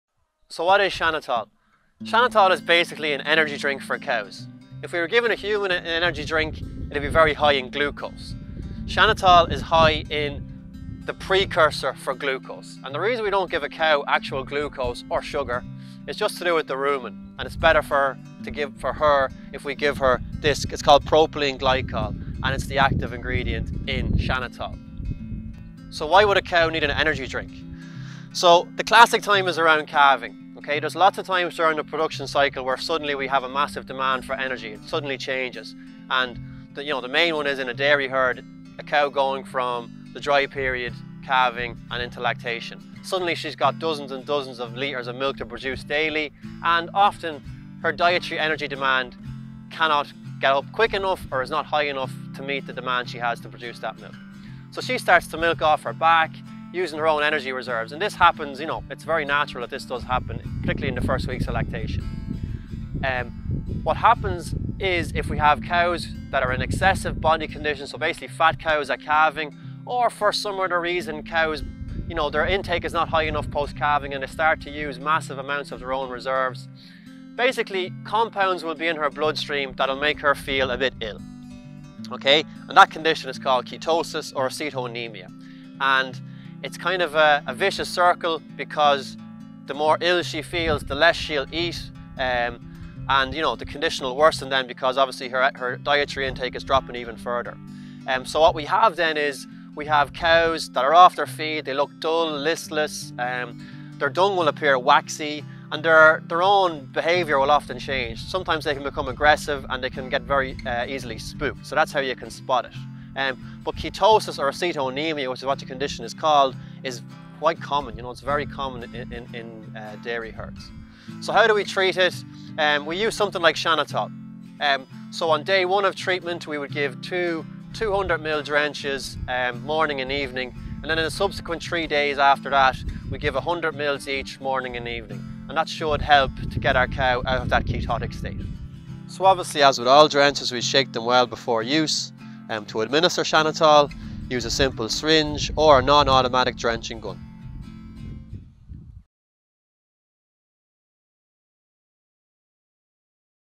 Chanatol
Chanatol is like an energy drink for cows and can be used to treat ketosis. Drench with Chanatol, 200ml morning and evening on day 1, and 100ml morning and evening for 3 more days.